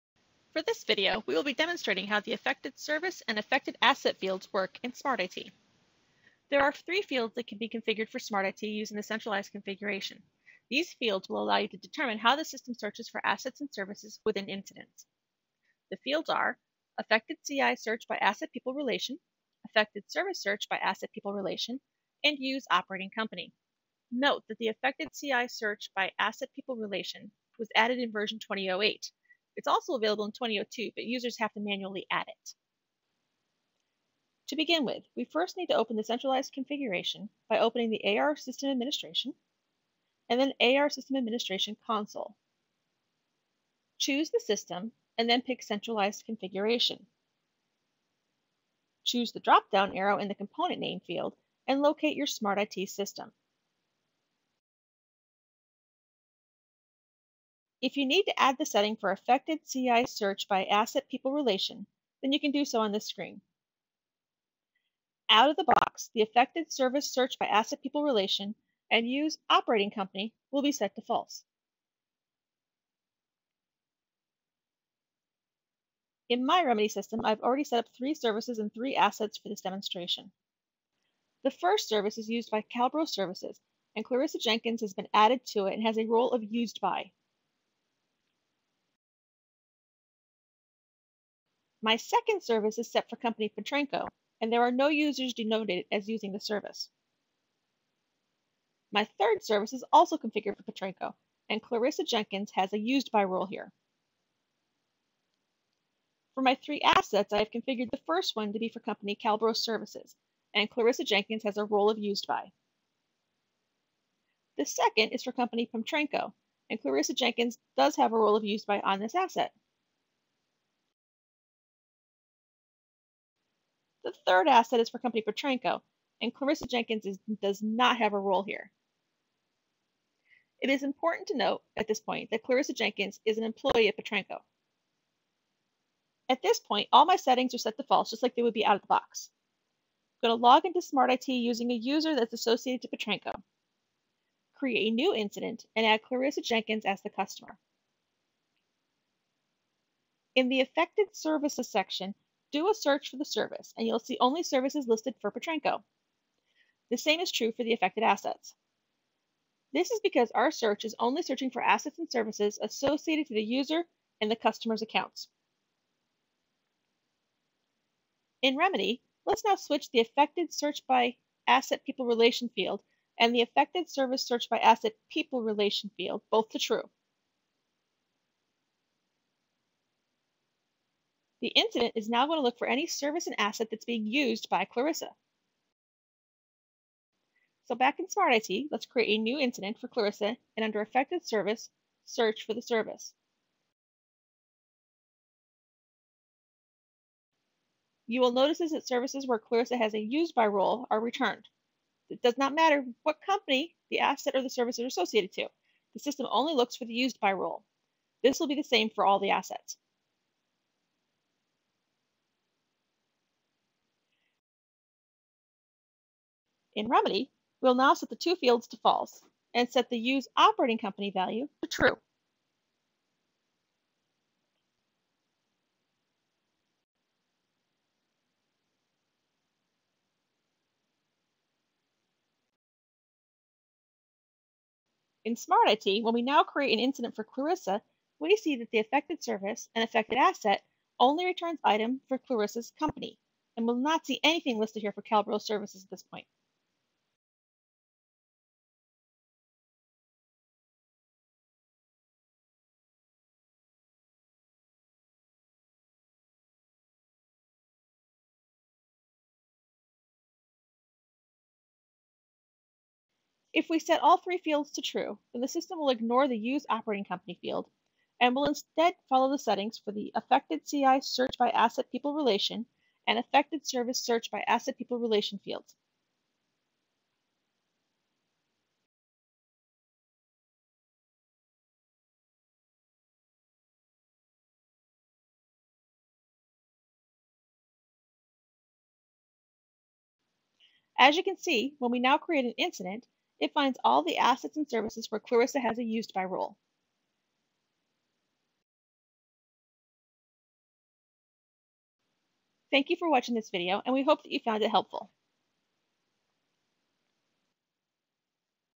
BMC Smart IT: How to Set Up Affected Service and Affected Asset Fields
There are 3 fields:
• affectedCISearchByAssetPeopleRelation
• affectedServiceSearchByAssetPeopleRelation
• UseOperatingCompany
That can be configured for Smart IT that will allow you to determine how the system searches for assets and services in incidents.
Smart IT,
affectedCISearch
ByAssetPeopleRelation,
affectedService
SearchByAsset
PeopleRelation,
UseOperatingCompany
BMC Support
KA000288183